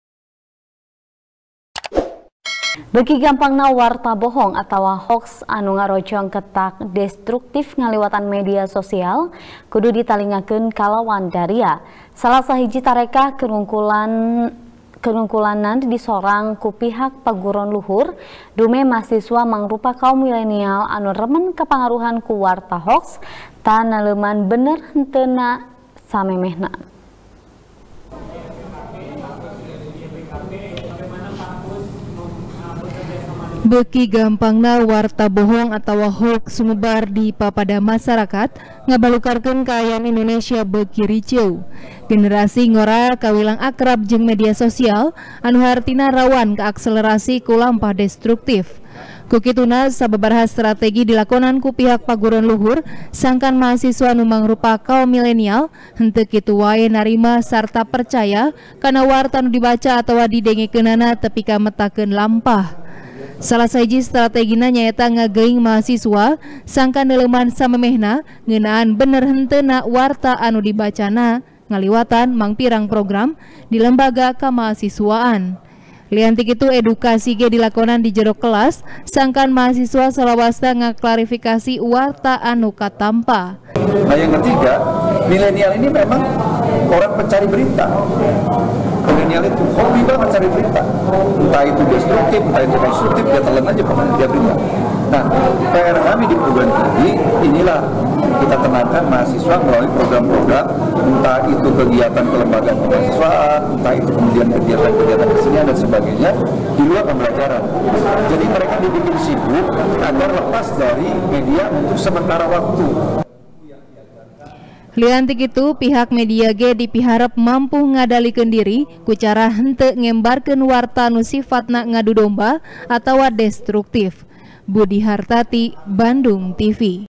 Kawas Kieu Strategi Keur Nyinglar Mahasiswa Tina Hoax | TP | BANDUNGTV
Salah Sahiji Tarekah Keur Ngungkulanana Disorang Ku Pihak Paguron Luhur, Dumeh Mahasiswa Mangrupa Kaom Milenial Anu Remen Kapangaruhan Ku Warta Hoax, Tan Neuleuman Bener Henteuna Samemehna.